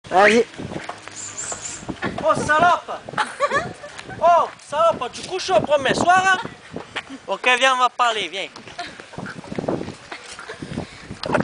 Troud à Marseille
Petit délire sur un concept arangé à la sauce marseillaise, la DRAGUE !